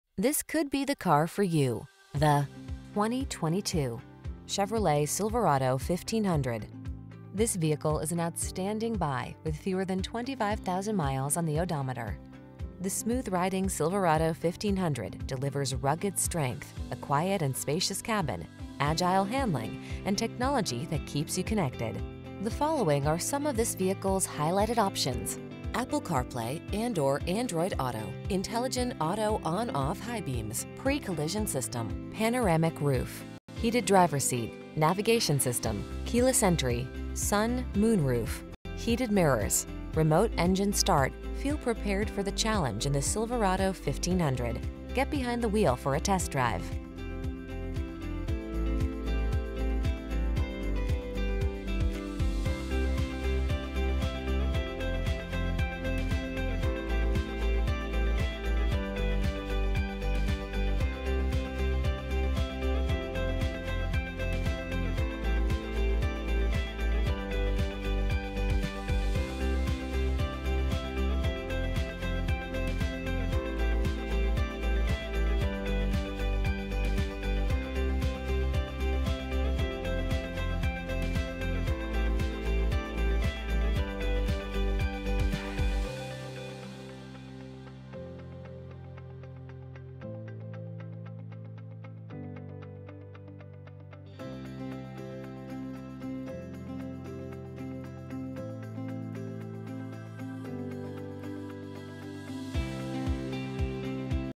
2022 Chevrolet Silverado_1500 Custom TX Bastrup, Camp Swift, Cedar Creek, Utley, Webberville
Pre-Owned 2022 Chevrolet Silverado_1500 Custom available at Covert Chevrolet Buick GMC located in 702 Texas 71, Bastrop, TX, 78602. Servicing the Bastrup, Camp Swift, Cedar Creek, Utley, Webberville area.

This could be the car for you, the  2022  Chevrolet Silverado_1500.  The smooth-riding Silverado 15 Hundred delivers rugged strength, a quiet and spacious cabin, agile handling, and technology that keeps you connected. The following are some of this vehicles highlighted options: Apple Carplay®/Android Auto®, Intelligent Auto on/off High Beams, Pre-Collision System, Panoramic Roof, Heated Driver Seat, Navigation System, Keyless Entry, Sun/Moonroof, Heated Mirrors, Remote Engine Start. Feel prepared for the challenge in the Silverado 15 hundred. Get behind the wheel for a test drive.